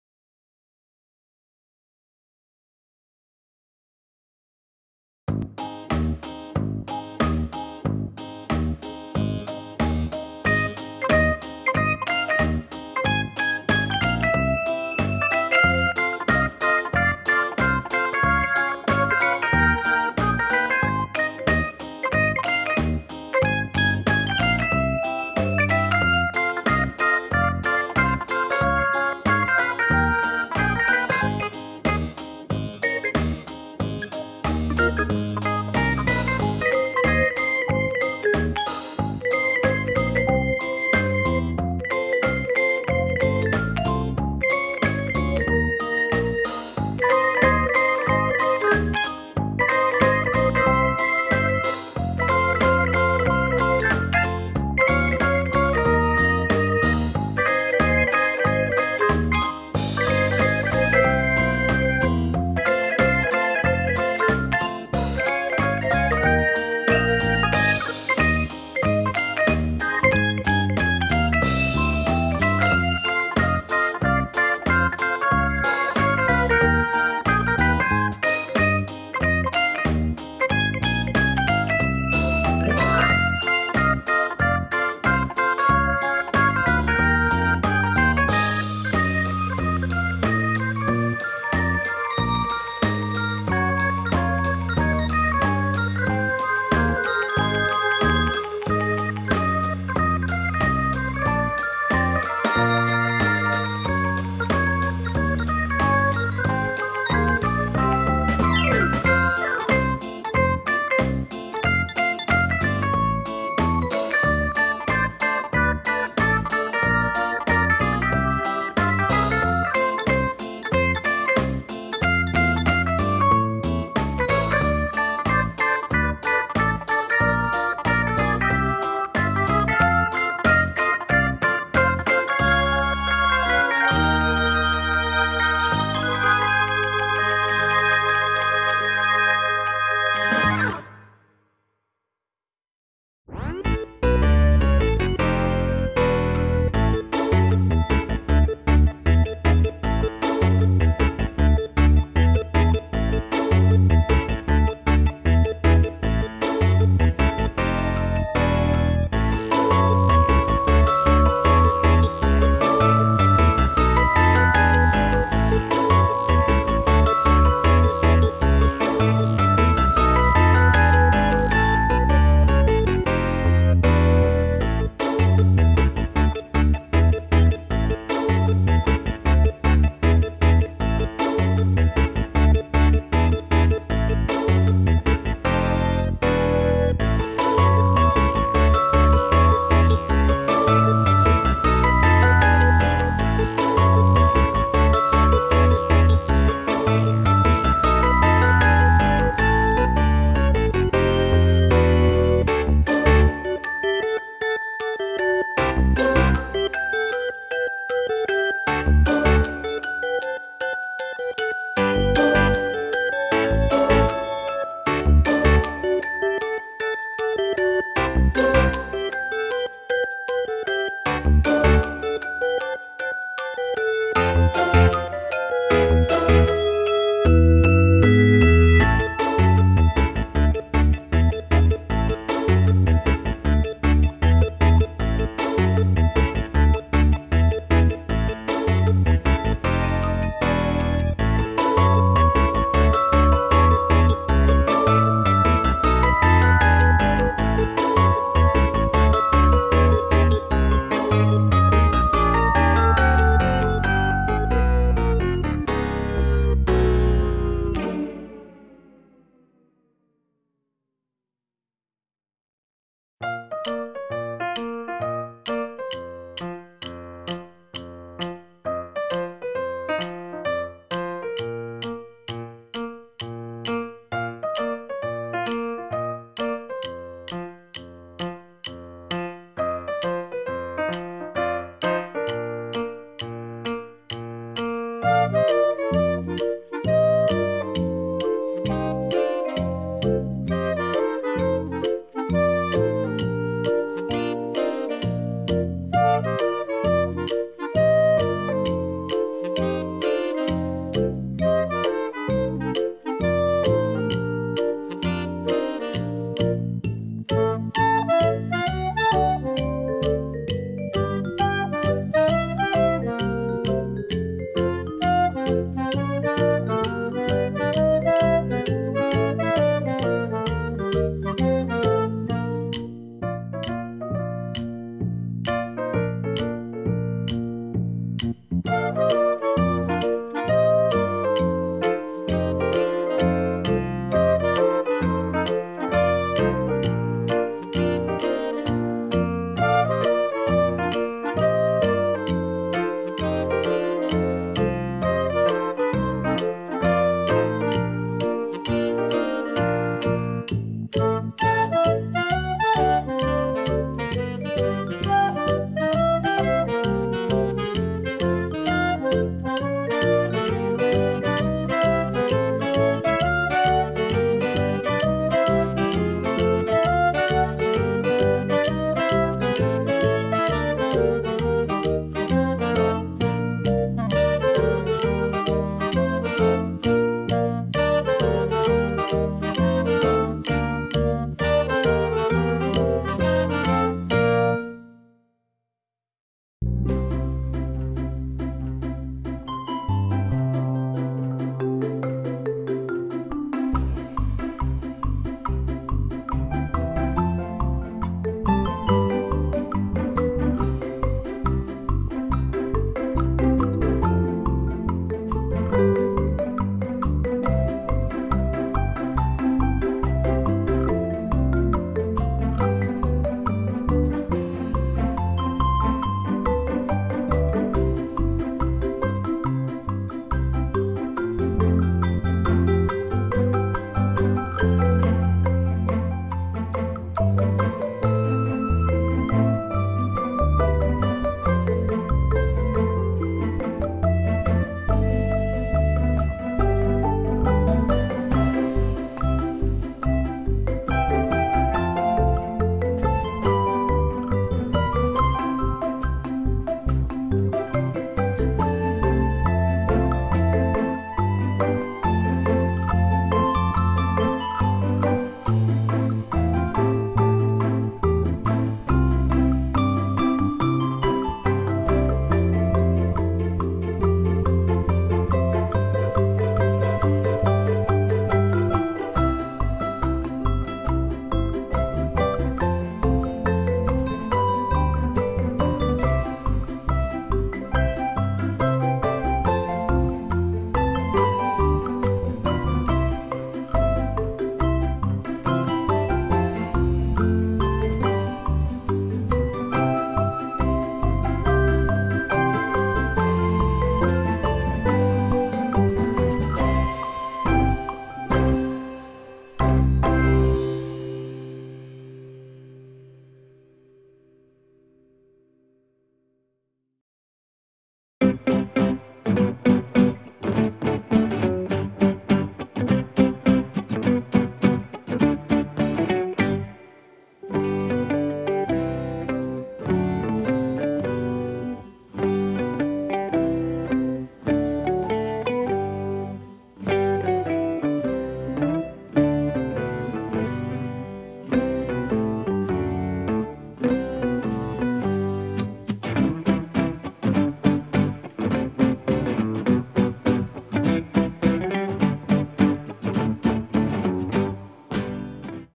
How to Draw Star and Heart ⭐ Doodle with Me 💖 Drawing & Coloring Page
Shooting star shining through the night. Hearts are the friends I like.